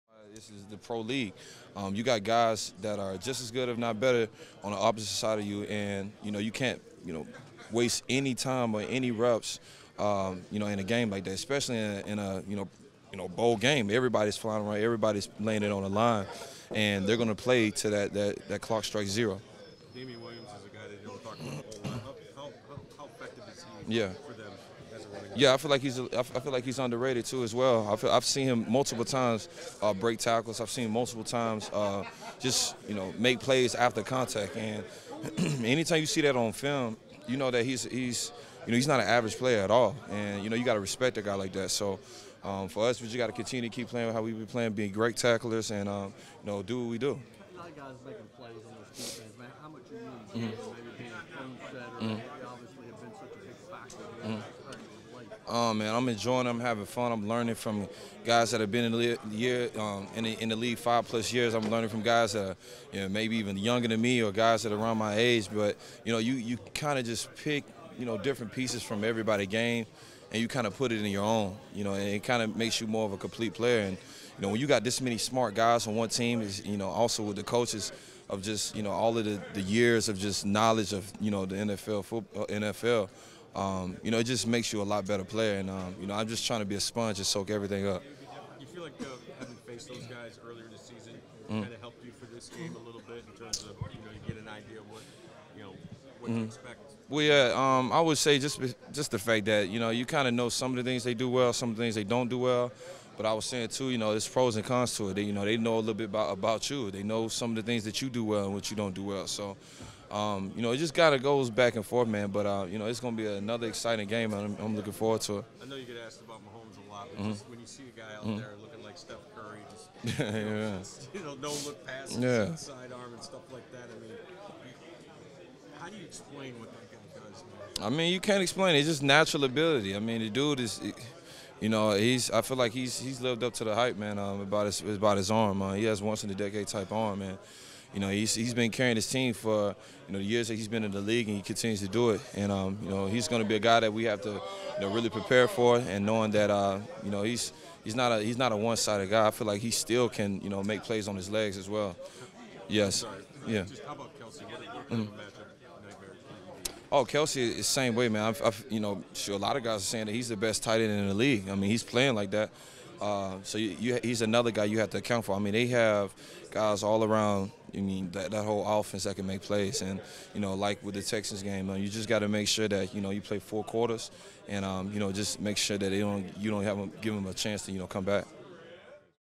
Rashaan Evans: Chiefs Have Guys All Around that Can Make Plays
Titans linebacker Rashaan Evans speaks with reporters Wednesday at Saint Thomas Sports Park. The Titans play the Kansas City Chiefs in the AFC Championship Game Sunday (2:05 p.m., CT) at Arrowhead Stadium.